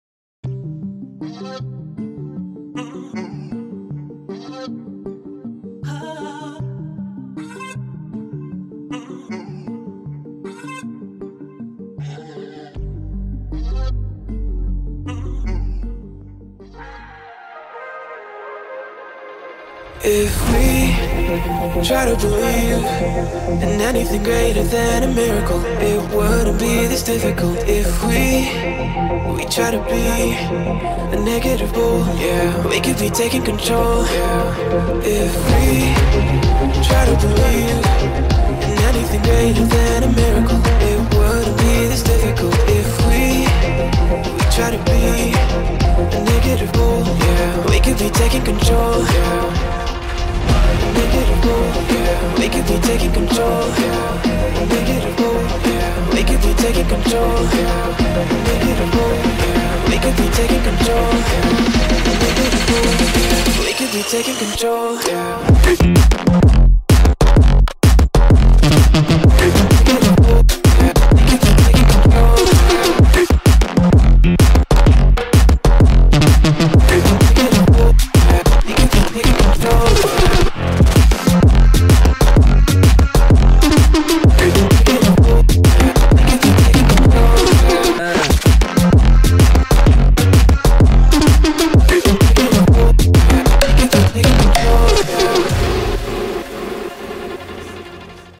Taking Control CBR 650 R
The Honda CBR650R is a four-cylinder sport bike made by Honda. It debuted at the 2018 EICMA in Milan, Italy as the successor to the CBR650F.
CBR 650 R is an ultra-sharp beast with sporting intent, fierce power, and even more exceptional torque. That's Honda CBR650R draws its inspiration from the CBR1000RR Fireblade, this incredible machine born on the racetrack, has carved its own niche among the free-spirited ones who love to go full throttle.